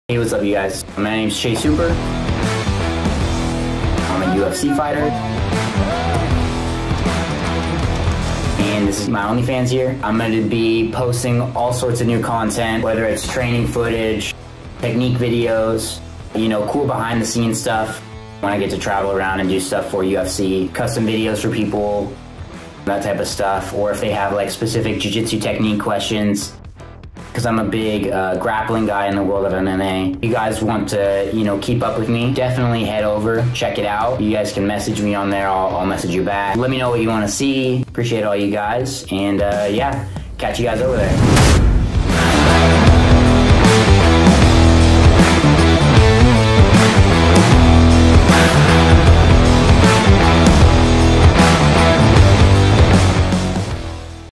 Keep up with UFC Fighter, Chase Hooper on OnlyFans!
What's it take to be the youngest UFC fighter in history?